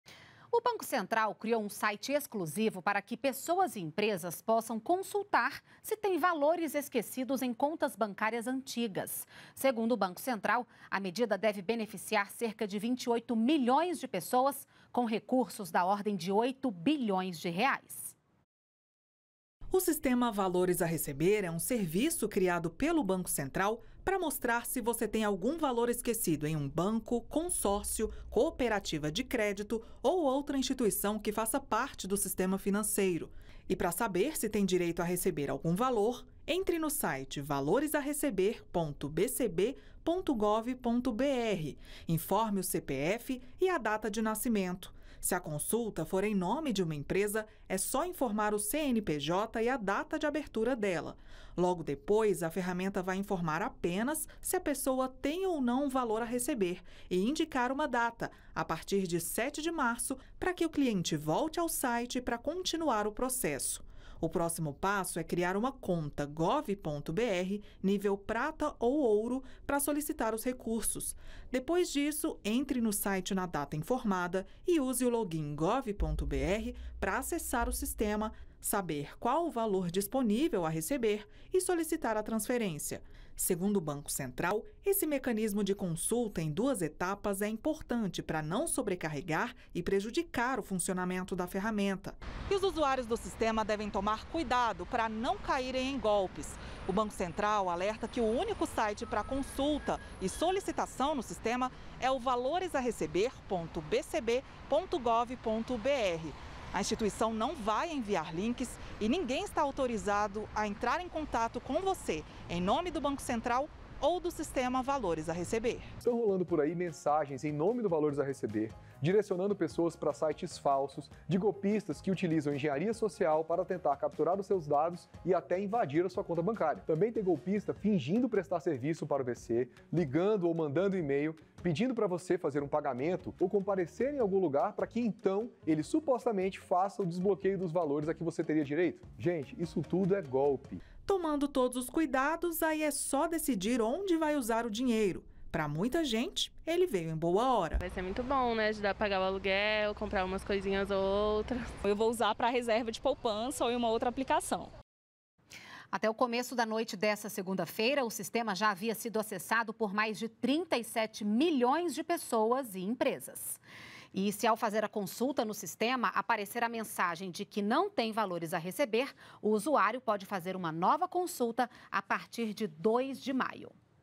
Economia | Valores a receber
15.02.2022 - O Banco Central lançou um site exclusivo para que pessoas e empresas recebam valores esquecidos. Medida deve beneficiar cerca de 28 milhões de pessoas, com recursos da ordem de R$ 8 bilhões.